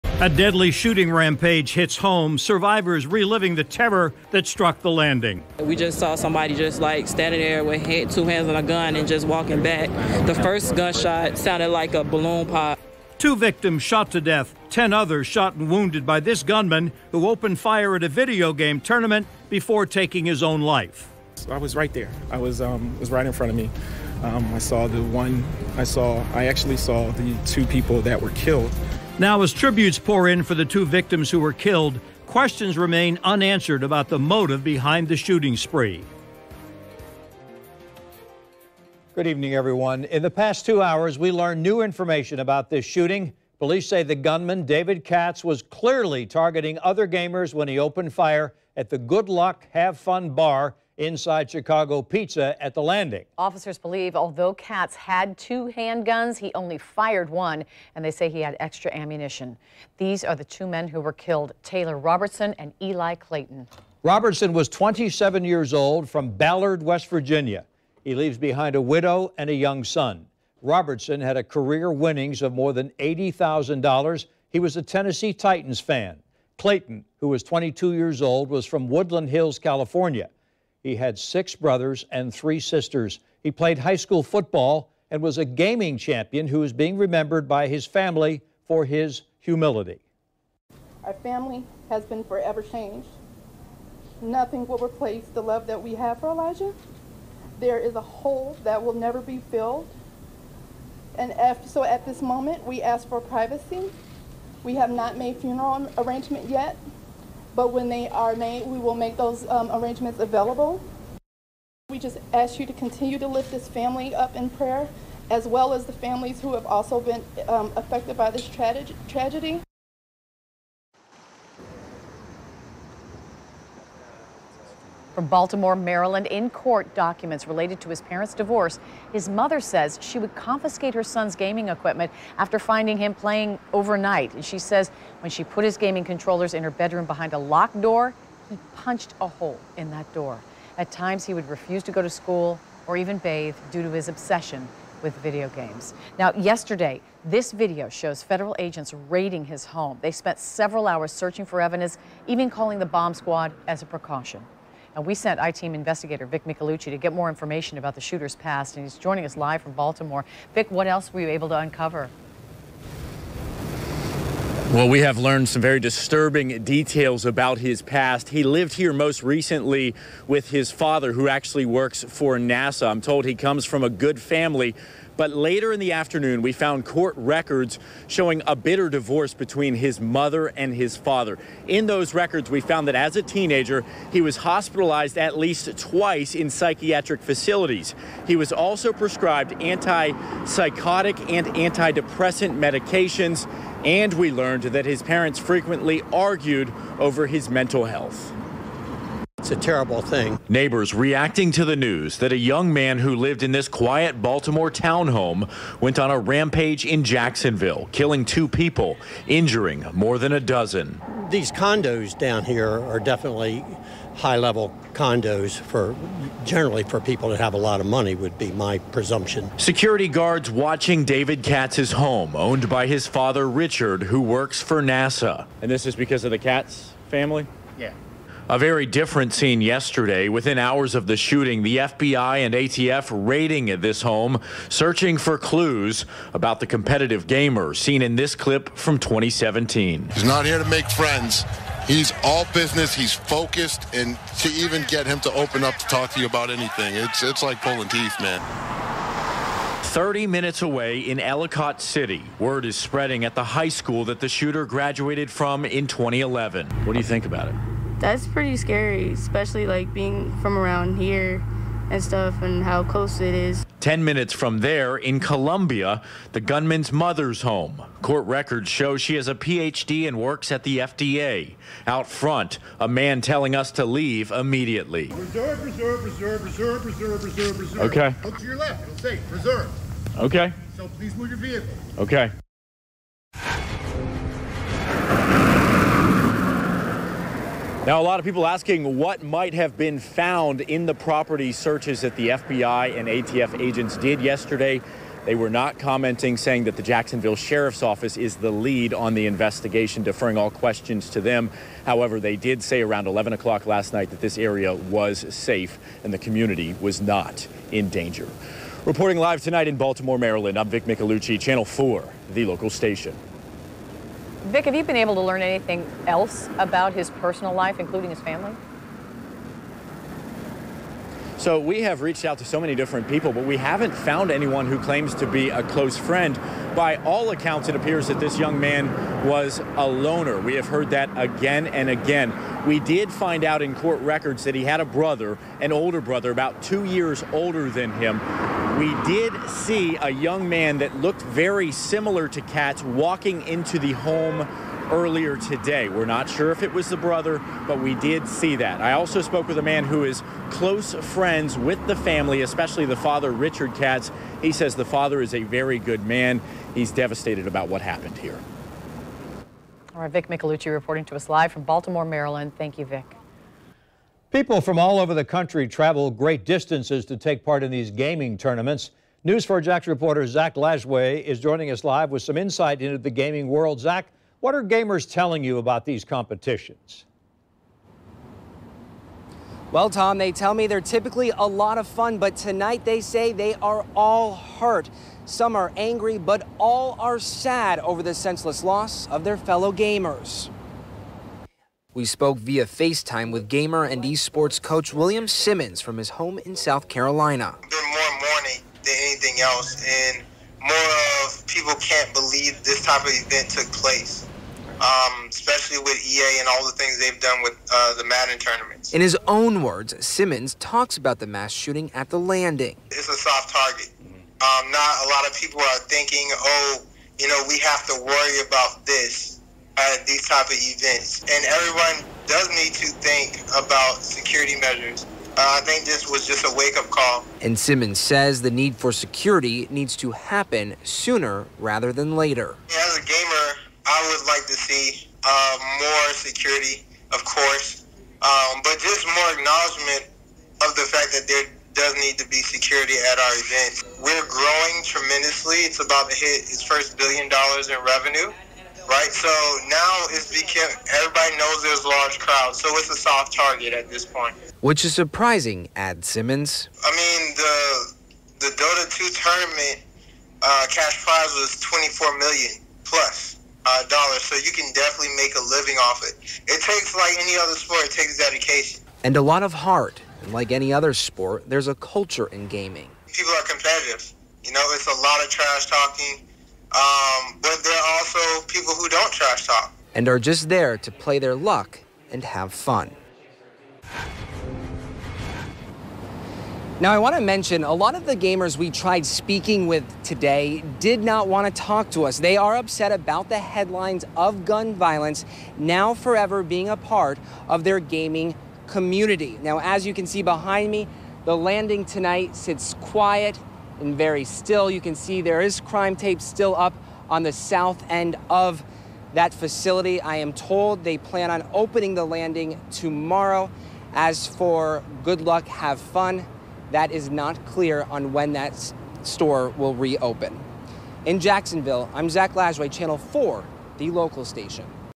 'Madden' gunman David Katz twice hospitalized for mental health
VIDEO: The 24-year-old man accused of opening fire at a "Madden NFL 19" regional tournament at the Jacksonville Landing, killing two and shooting 10 others, clearly targeted fellow gamers, Sheriff Jacksonville Sheriff Mike Williams announced.